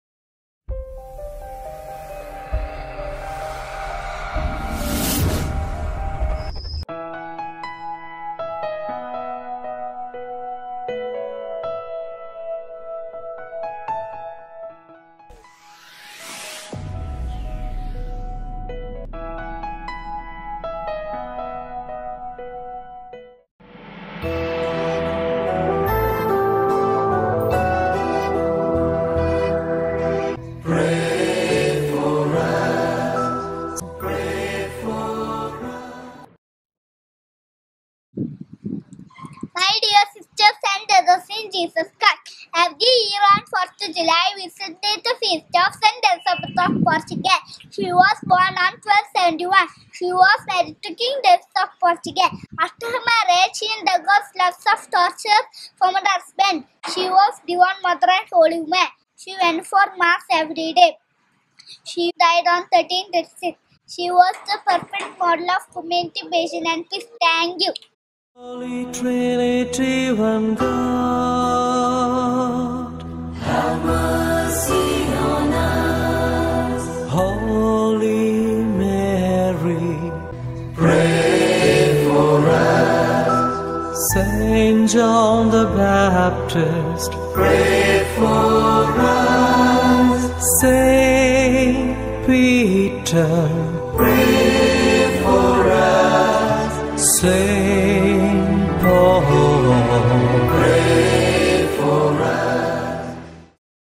July 4 - Saint Elizabeth of Portugal
Daily Saint Series by catechism children of St Jude Sunday School Udayanagar. This video is about Saint Elizabeth of Portugal, Elizabeth of Aragon, more commonly known as Saint Elizabeth of Portugal, (1271 – 4 July 1336; Elisabet in Catalan, Isabel in Aragonese, Portuguese and Spanish), was queen consort of Portugal, a tertiary of the Franciscan Order and is venerated as a saint of the Catholic Church.She was able to establish for herself a pattern of life conducive to growth in God’s love, not merely through her exercises of piety, including daily Mass, but also through her exercise of charity, by which she was able to befriend and help pilgrims, strangers, the sick, the poor—in a word, all those whose need came to her notice. At the same time she remained devoted to her husband, whose infidelity to her was a scandal to the kingdom.